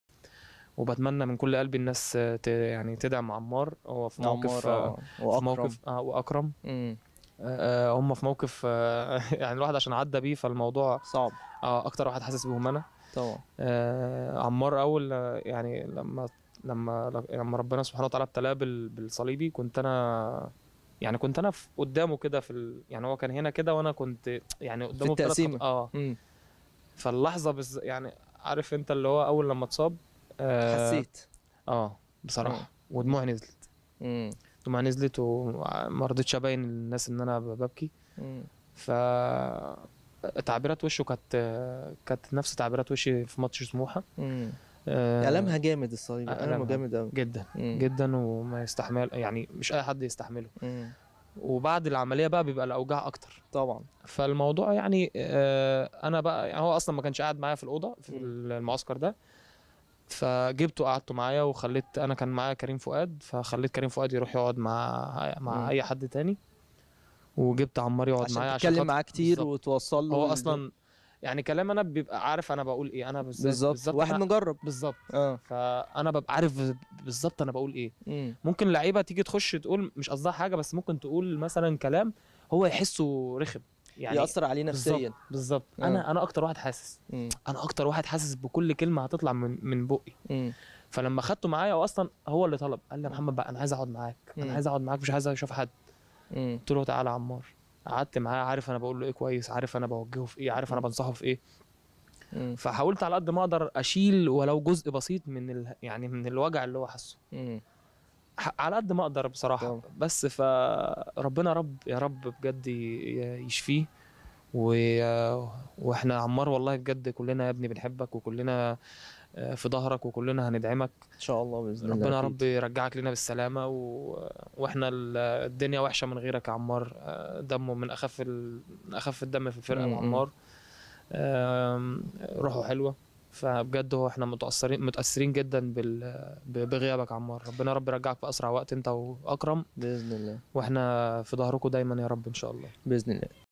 محمود يطلب من جماهير الأهلي الوقوف خلف الثنائي عمار حمدي وأكرم توفيق ودعمهم | الأهلي في المونديال
محمد محمود يطلب من جماهير الأهلي الوقوف خلف الثنائي عمار حمدي وأكرم توفيق ودعمهم | الأهلي في المونديال